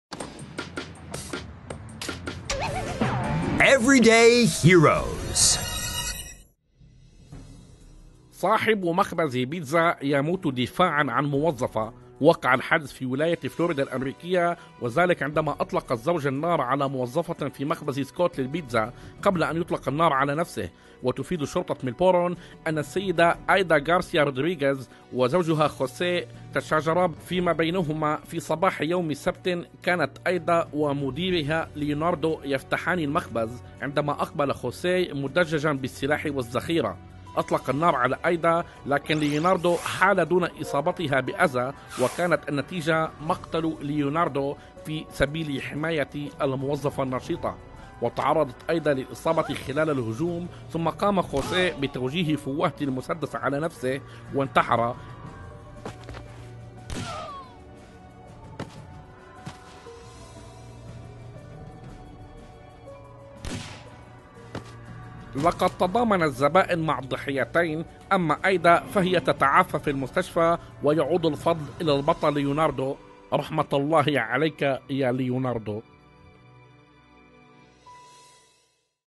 صاحب مخبز بيتزا يلقى حتفه في سبيل الدفاع عن إحدى الموظفات من زوجها المسلّح
صاحب مخبز بيتزا يموت دفاعاً عن موظفة
وقع الحادث في ولاية فلوريدا الأمريكية... وذلك عندما أطلق الزوج النارعلى موظفة في مخبز سكوت للبيتزا, قبل أن يطلق النار على نفسه
وتفيد شرطة ميلبورن أن السيدة أيدا غارثيا رودريغز وزوجها خوسي تشاجرا فيما بينهما.
في صباح يوم سبت كانت أيدا ومديرها ليوناردو يفتحان المخبز... عندما أقبل خوسي مدججاً بالسلاح والذخيرة
أطلق النار على أيدا لكن ليوناردو حال دون إصابتها بأذى... وكانت النتجية مقتل ليوناردو في سبيل حماية موظفته
وتعرضت أيدا للإصابة خلال الهجوم.. ثم قام خوسي بتوجيه فوهة المسدس على نفسه وانتحر
لقد تضامن الزبائن مع الضحيتين... أما أيدا فهي تتعافى في المستشفى ويعود الفضل إلى البطل ليوناردو.